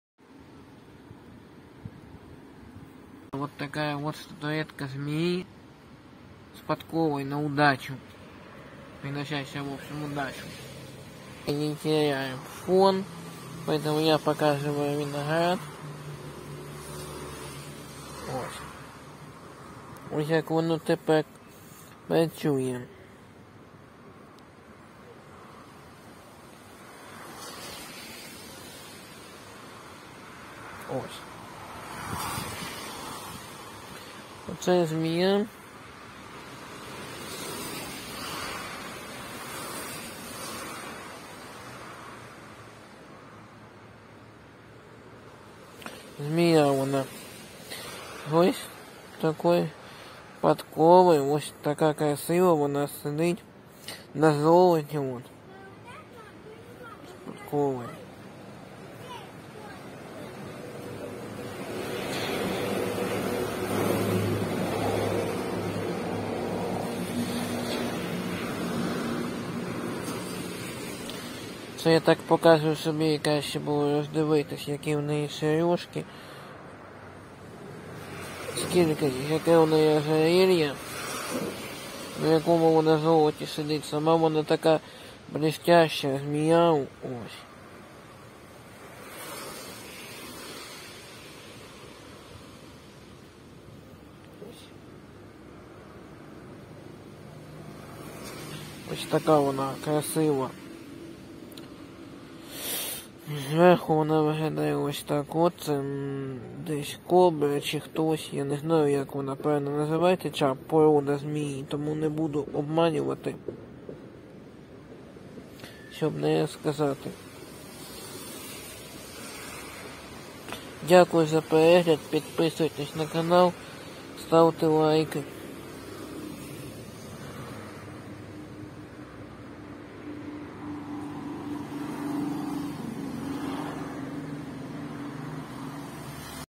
Статуэтка Змея
Статуэтка Змея следящая на подкове приносящая удачу!